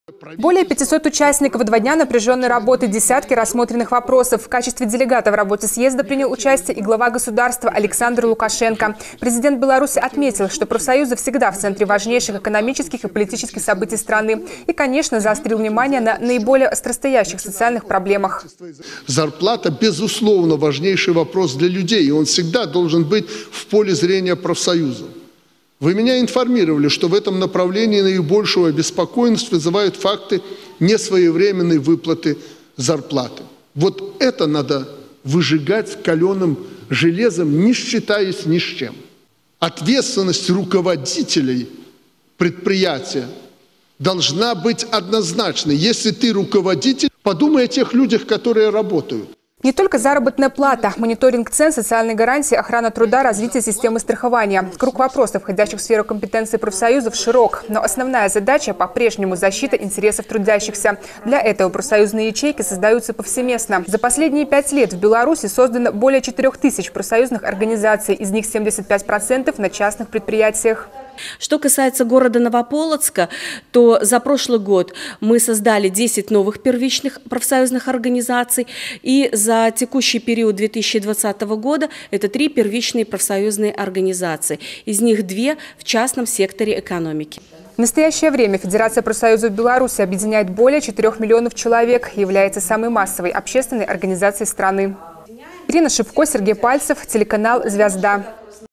В конце февраля в Минске прошёл 8-й Съезд Федерации профсоюзов Беларуси
По горячим следам. В конце февраля в Минске прошёл 8-й Съезд Федерации профсоюзов Беларуси. На повестке дня – подведение итогов работы за прошедшие пять лет и задачи на следующую пятилетку. На съезд было делегировано 4 представителя от Новополоцка и по итогам съезда уже в Новополоцке состоялось заседание городского объединения профсоюзов. Основная цель – донести сведения до тех, кто не присутствовал в столице.